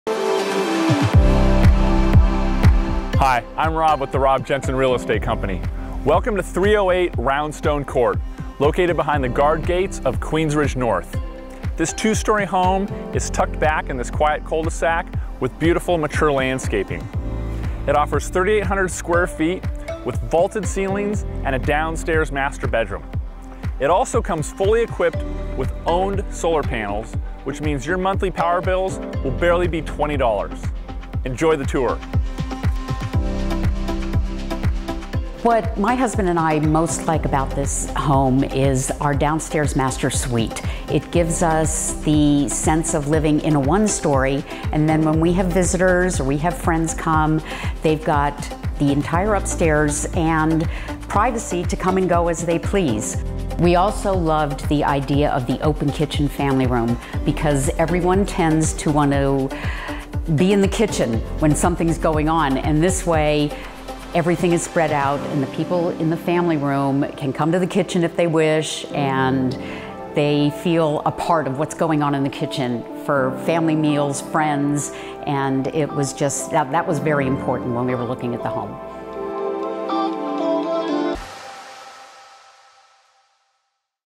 Luxury Home Tour | Solar-Powered Estate in Las Vegas | 308 Round Stone Court
Enjoy this luxury home tour of 308 Round Stone Court, located in the 24-hour guard gated community of Queensridge within Las Vegas, Nevada. A major highlight of this home is its 39-panel solar PV system. An upgrade worth over $37,000, this system provides usable solar energy and over $2,000 in annual energy savings. Tray and vaulted ceilings, Tuscan columns, and sunken living spaces create a sense of architectural intrigue in this home.

Sold on March 25, 2019 for $655,000 at $172/SF
3,798 SF | 4 BR | 3 Full BA

We specialize in working with buyers and sellers in guard gated communities in Summerlin, Las Vegas, and Henderson.